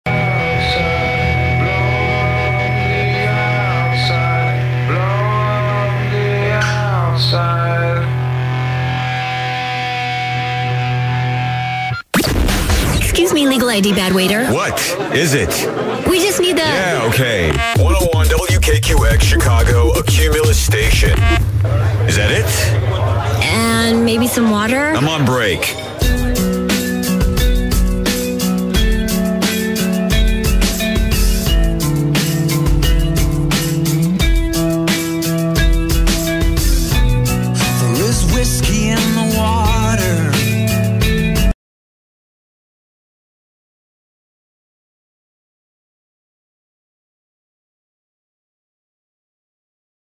WKQX Chicago IL Q101 101.1 LEGAL ID ID 8-4-15
WKQX FM Q101 Chicago Legal ID "bad waiter" Recorded 8-4-15